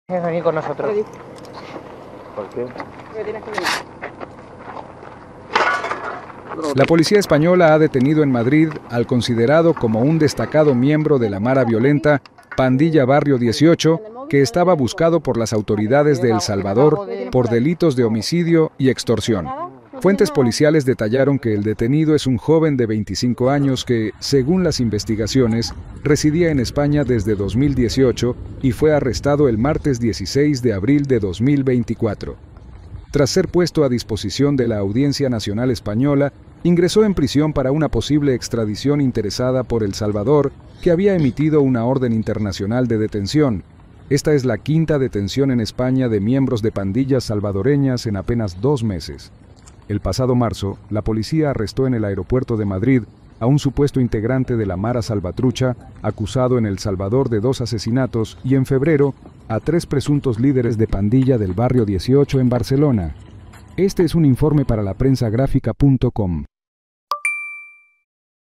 Líder de la pandilla Barrio 18 buscado por El Salvador es detenido en España
La Policía española ha detenido en Madrid al considerado como un destacado miembro de la mara violenta Pandilla Barrio 18 que estaba buscado por las autoridades de El Salvador por delitos de homicidio y extorsión.